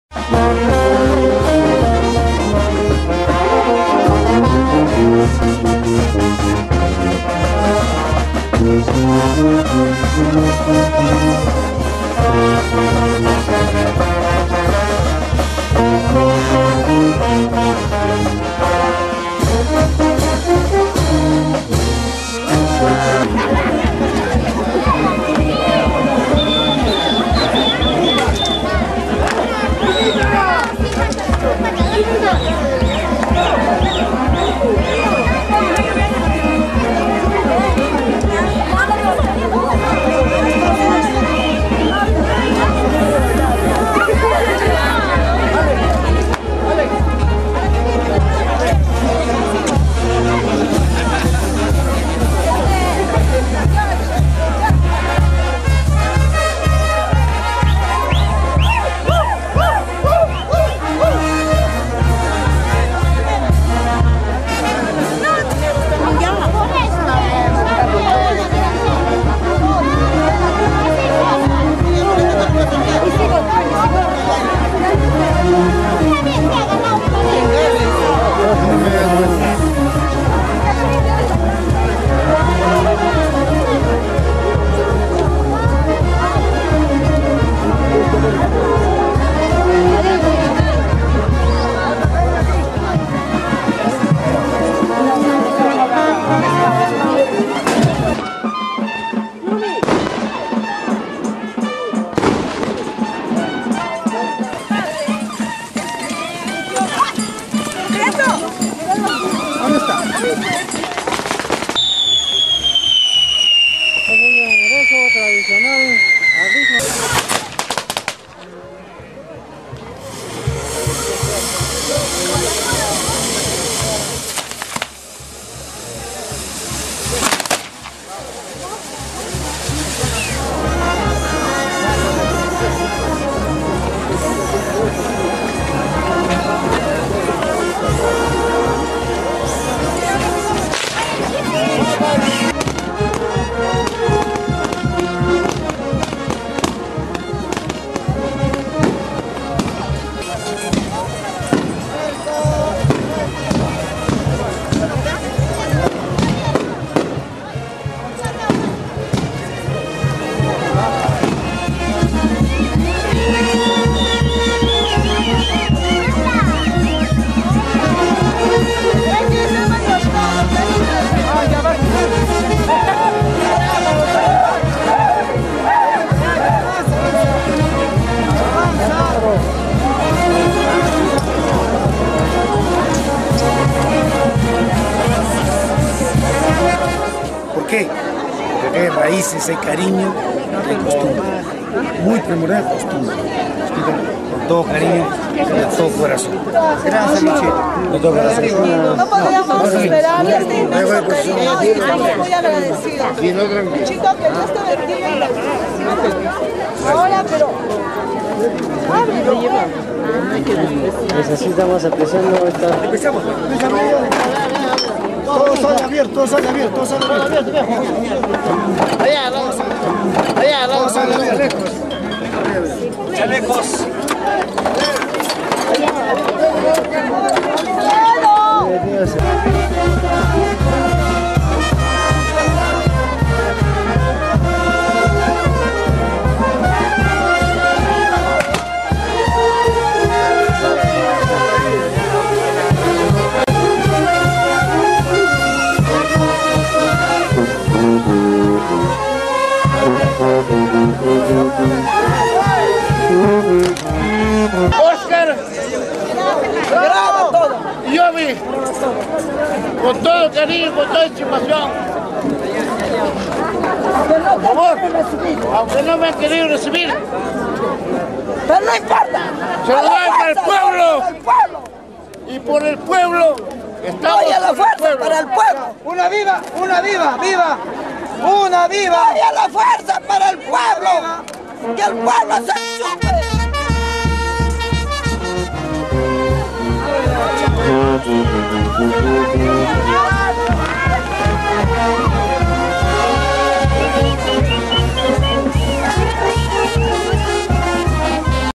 Chacapampa 2010 , Mayordomo Sr. Oscar García. Perù- 09
Chacapampa 2010. Fiesta de la Santísima Virgen de la Candelaría. Mayodomo Sr. Oscar García y esposa Sra. Yobe Luya de García.